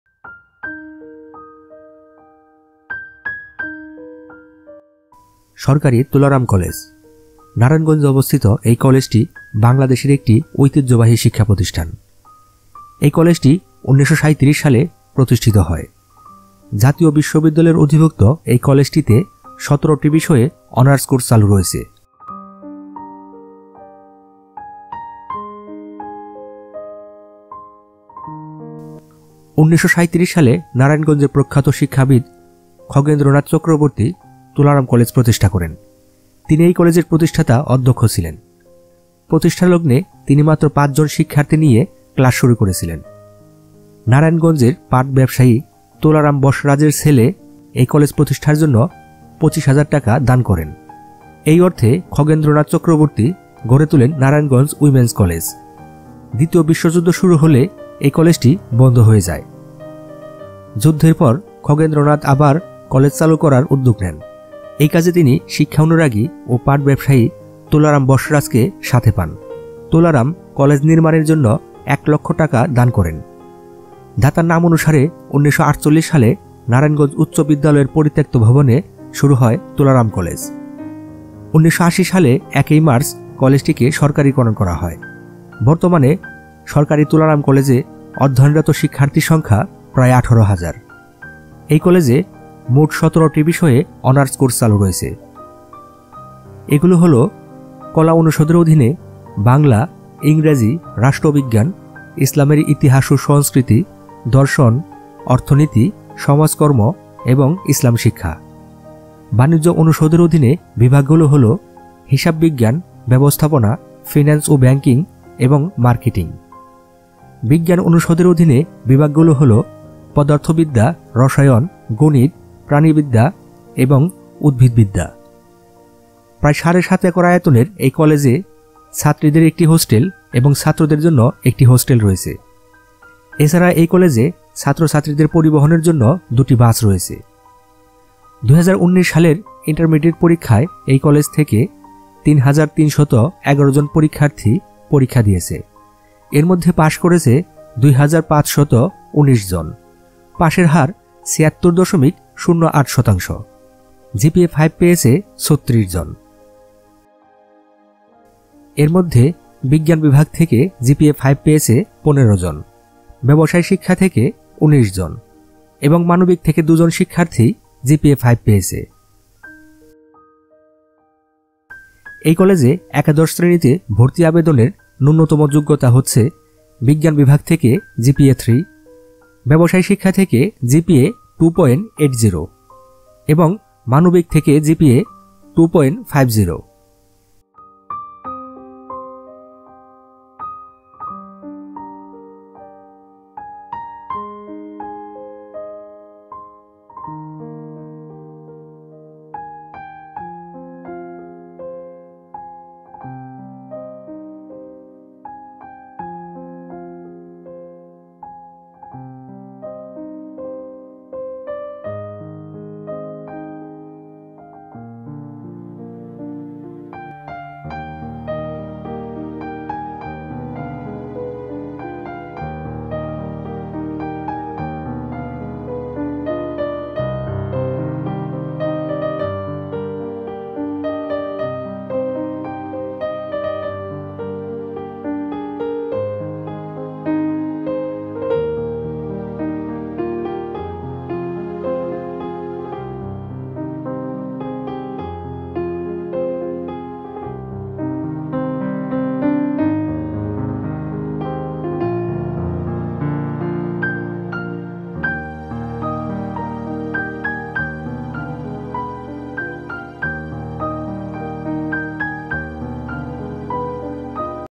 সরকারি তোলারাম কলেজ,নারায়ণগঞ্জ। Govt. Tolaram College। Campus View, History, Admission information.
সরকারী তোলারাম কলেজ, নারায়ণগঞ্জে অবস্থিত এই কলেজটি বাংলাদেশের একটি ঐতিহ্যবাহী শিক্ষা প্রতিষ্ঠান।এই কলেজটি ১৯৩৭ সালে প্রতিষ্ঠিত হয়। কলেজটি জাতীয় বিশ্ববিদ্যালয়ের অধিভুক্ত।এই কলেজটি তে ১৪টি বিষয়ে অনার্স কোর্স চালু আছে জাতীয় বিশ্ববিদ্যালয়ের অধীনে।

১৯৩৭ সালে নারায়ণগঞ্জের প্রখ্যাত শিক্ষাবিদ খগেন্দ্র নাথ চক্রবর্তী তোলারাম কলেজ প্রতিষ্ঠা করেন। তিনি এই কলেজের প্রতিষ্ঠাতা অধ্যক্ষ ছিলেন।১৯৮০ খ্রিস্টাব্দের ১ মার্চ এই কলেজটিকে জাতীয়করণ করা হয়। বর্তমানে ‘সরকারি তোলারাম কলেজ’- এ অধ্যয়নরত শিক্ষার্থীদের সংখ্যা প্রায় ১৮০০০ জন।

Government Tolaram College is now Government Tolaram University College. Tolaram College Narayanganj, Bangladesh was founded in 1937 by Khagendro Nath Chokroborti, who also served as its first principal. The college had an initial intake of five students. It is situated at the center of Narayanganj near Chashara. The college offers honors and masters program and is governed under the authority of the National University of Bangladesh.